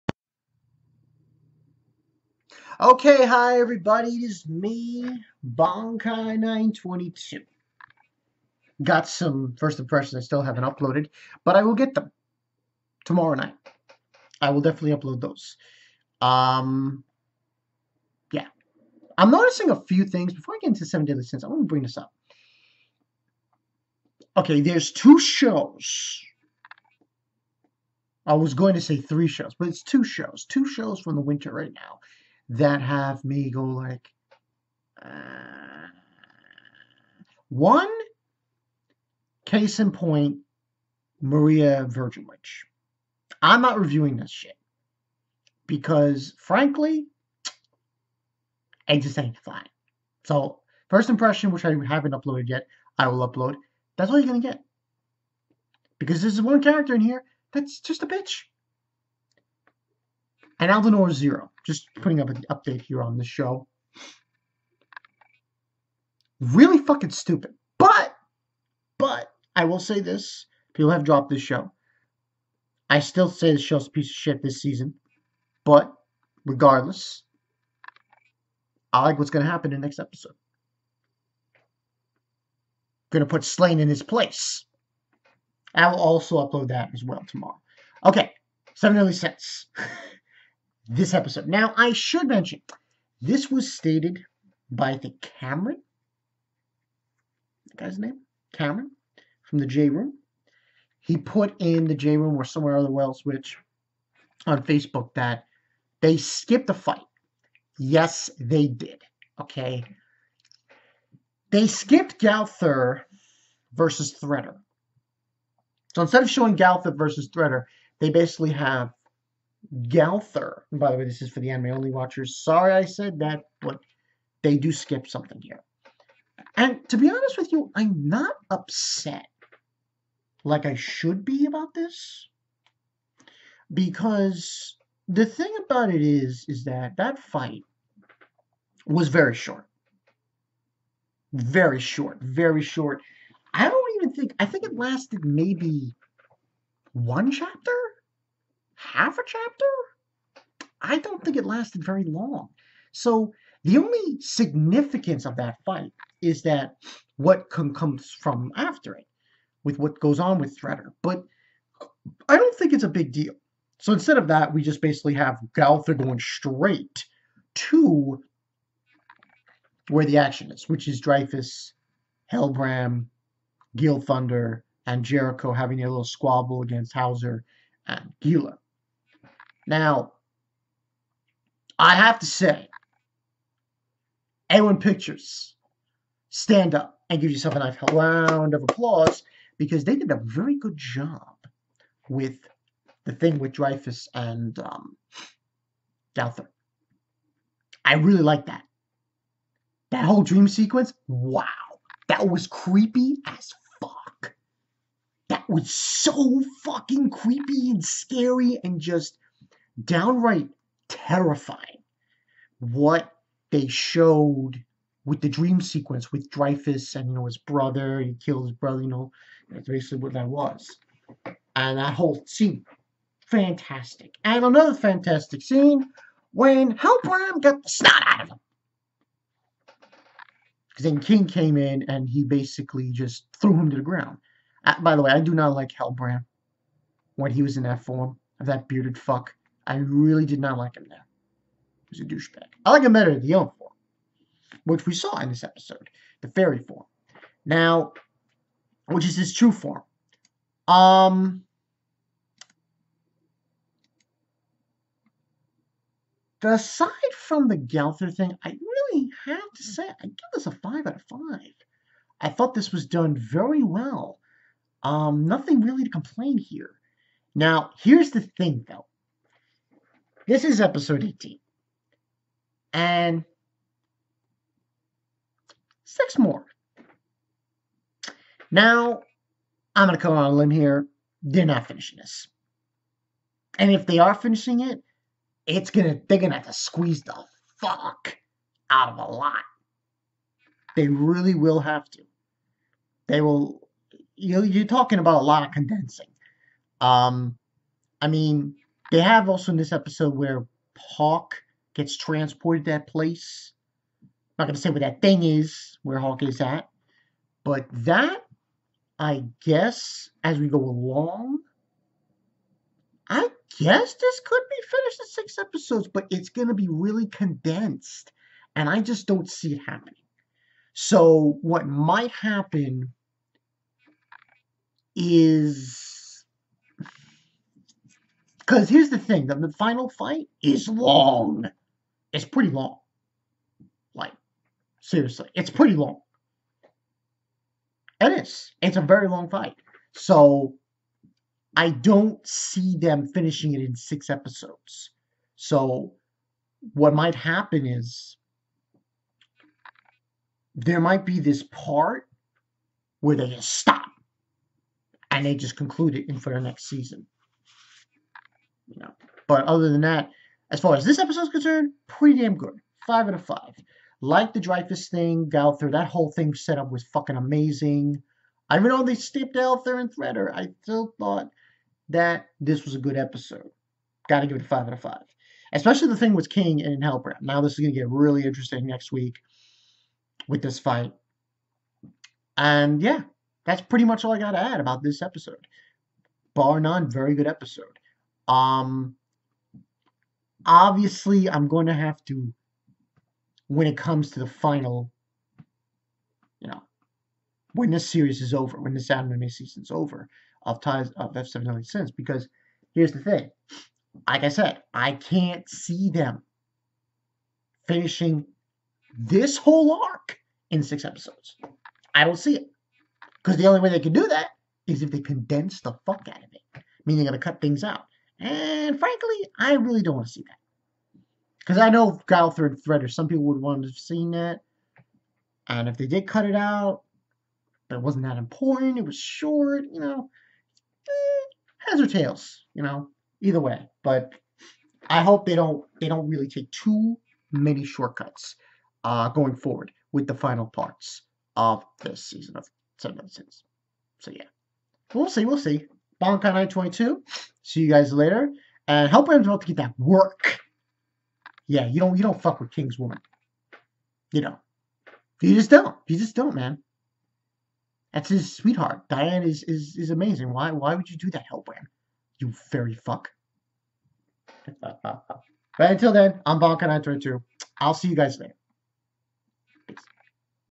SEVEN DEADLY SINS EPISODE 18 REVIEW-GOWTHER INDUCING NIGHTMARE AND KING KICKS SOME HELBRAM ASS!!!!
Great episode definitely like what is going down but the pacing is pretty fast how will they finish this in 6 episodes we shall wiat and see!!! also that dream sequence freaking goat!